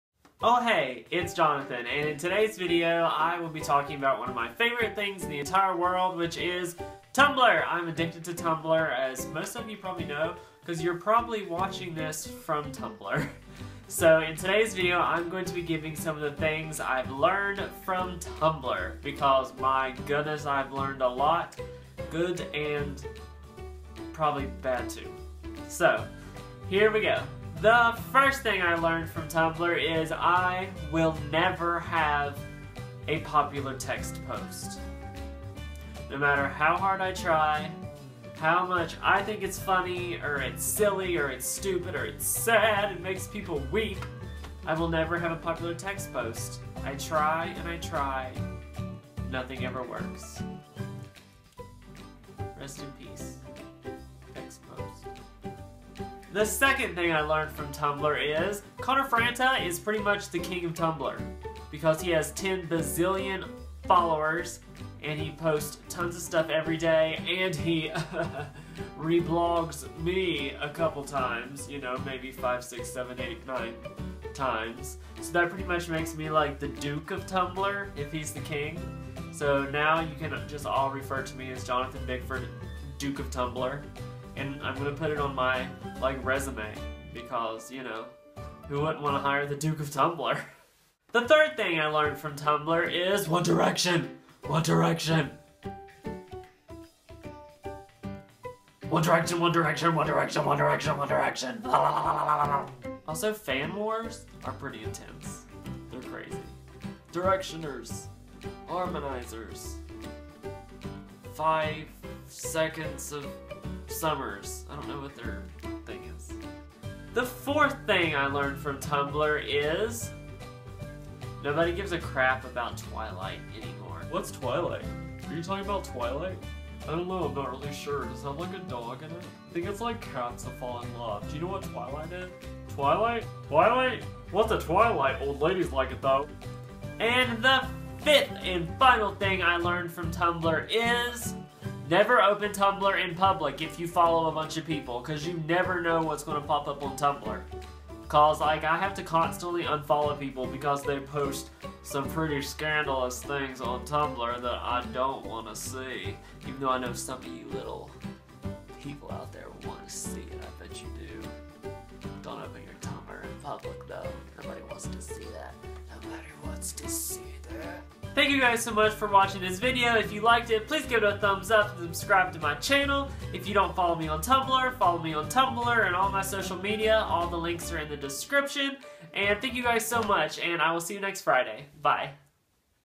THINGS I LEARNED FROM TUMBLR!
In this video, I talk about some of the things I learned from my favorite social media site Tumblr!  I hope you like it, and it makes you laugh!

Vine: JonathanOhHey